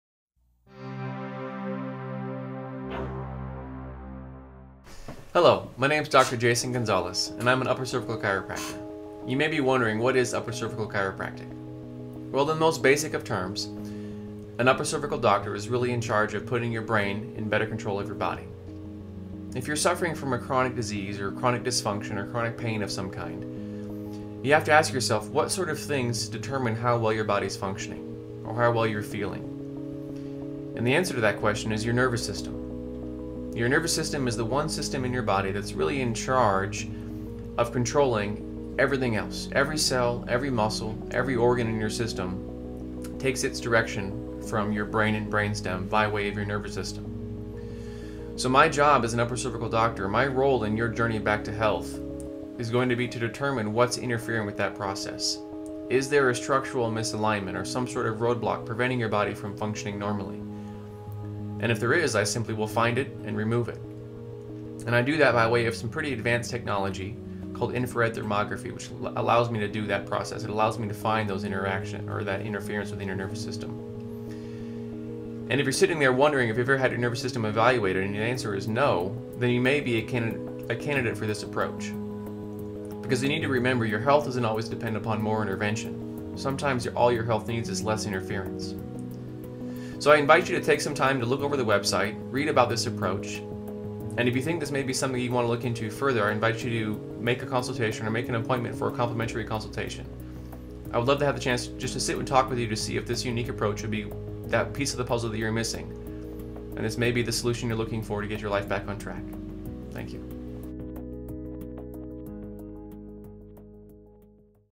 Dr J
Dr. Jason Gonzales offers a possible solution through the practice of upper cervicle chiropractic services.